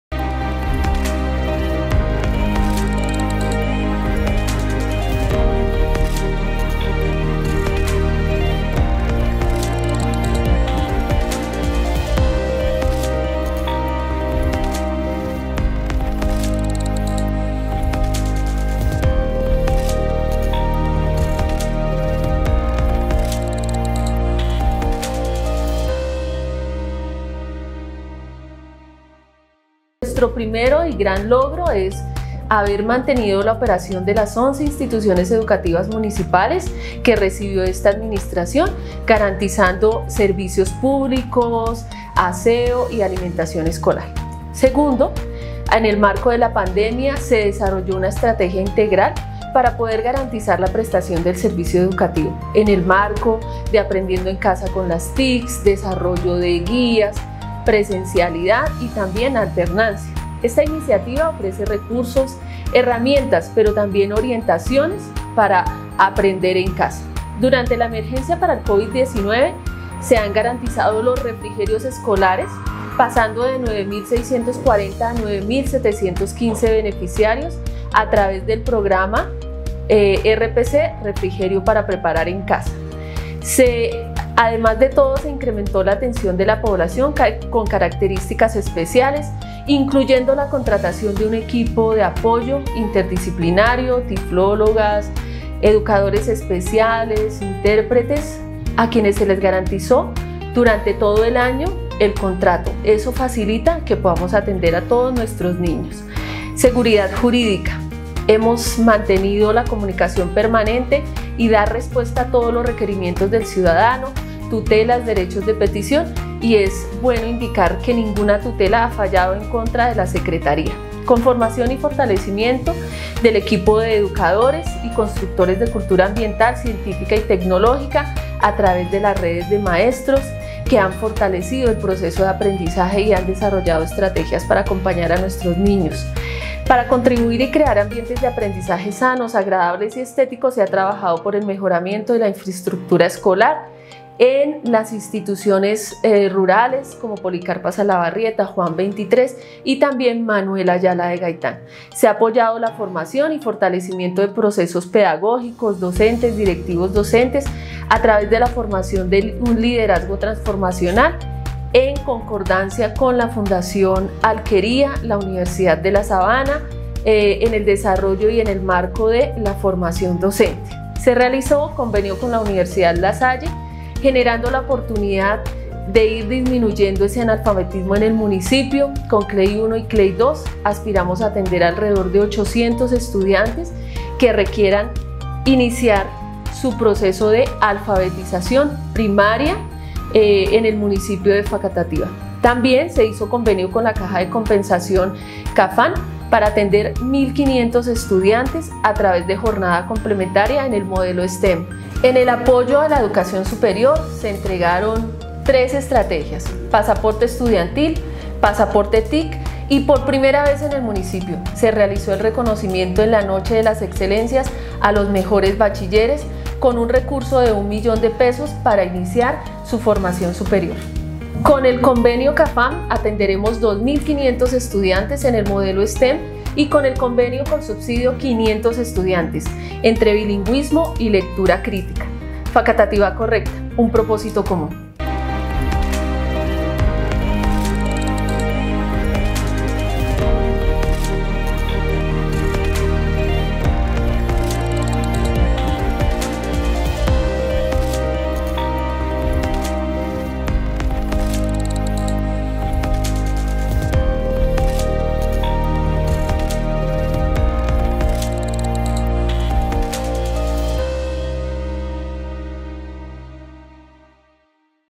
La Secretaría de Educación presenta un informe de los logros alcanzados a lo largo del presente año
Educación ✅ || La Secretaría de Educación presenta un informe de los logros alcanzados a lo largo del presente año
El Señor Alcalde Guillermo Aldana Dimas, a través de Mary Luz Bermúdez, Secretaria de Educación de Facatativá, en el marco de proceso de rendición de cuentas a la comunidad, presenta un balance de los logros alcanzados por la entidad en materia de educación a niños, jóvenes y población en condición de discapacidad del municipio; así como de capacitación continua a docentes y personal directivo docente.
- Mantener operaciones en las 11 instituciones educativas municipales, garantizando la prestación de servicios públicos, aseo y alimentación escolar.
- En el marco de la pandemia se desarrollaron estrategias de aprendizaje para garantizar el derecho a la educación a través de las TIC y mediante el uso de guías y realizando orientación.
- En materia del Plan de Alimentación Escolar, se amplió el número de beneficiarios, pasando de 9.640 a 9.715 estudiantes.
- Robustecimiento del programa de educación a población en condición de discapacidad con personal interdisciplinario idóneo con atención a lo largo del año.
- Atención a las dudas, requerimientos, quejas, derechos de petición y tutelas, de estas últimas, ninguna con fallo en contra de la Secretaría de Educación.
- Conformación y fortalecimiento del equipo de educadores y constructores de cultura ambiental, científica y tecnológica, a través de las redes de maestros.
- Mejoramiento de la infraestructura escolar, especialmente en instituciones y sedes rurales.
- Apoyo en la formación y desarrollo de procesos pedagógicos a docentes y directivos docentes.
- Convenio con entidades de educación superior para disminuir el analfabetismo en educación primaria en el municipio y para la ampliación de la estrategia de jornada única.
- En materia de Educación superior, se han desarrollado estrategias como el pasaporte estudiantil, el pasaporte TIC y el reconocimiento a los mejores bachilleres con el bono de la excelencia.